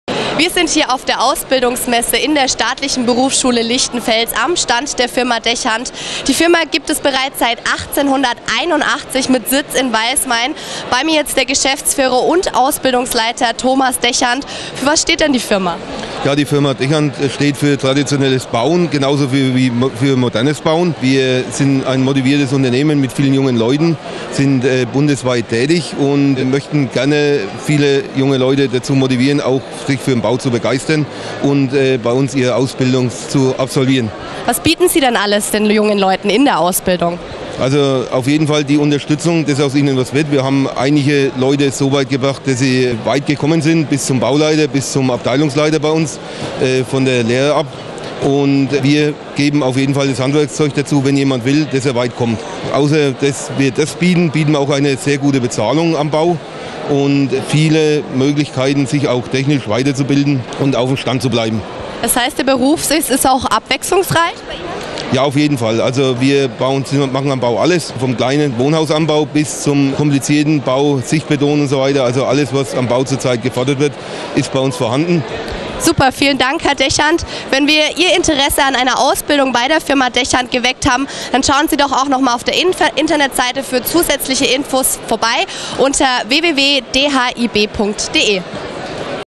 Azubimesse Lichtenfels 2017
Der Fernsehsender TVO besucht unseren Stand bei der Ausbildungsmesse in Lichtenfels. Im Interview gibt Ausbildungsleiter Thomas Dechant Einblicke, warum es sich lohnt, bei der dechant hoch- und ingenieurbau gmbh Karriere zu machen.